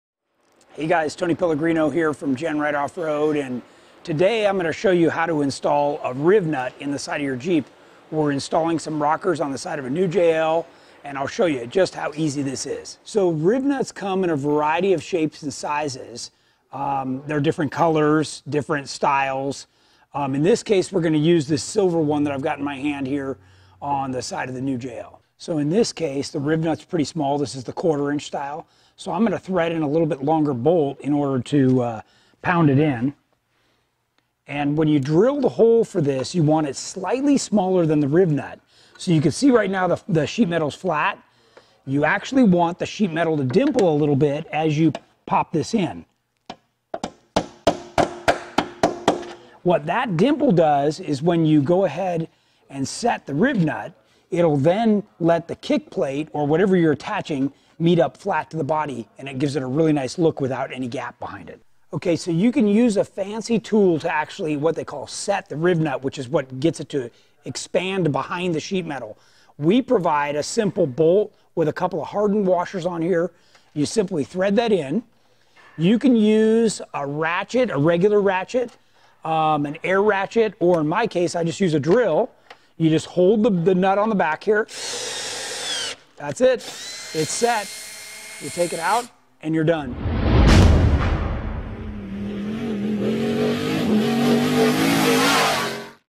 How To | Installing Rivnuts on your Jeep
Ever heard of a Rivnut? Pretty common for aftermarket Jeep Wrangler fenders, rocker guards, and more. Here we give you some quick tips to make installation easy.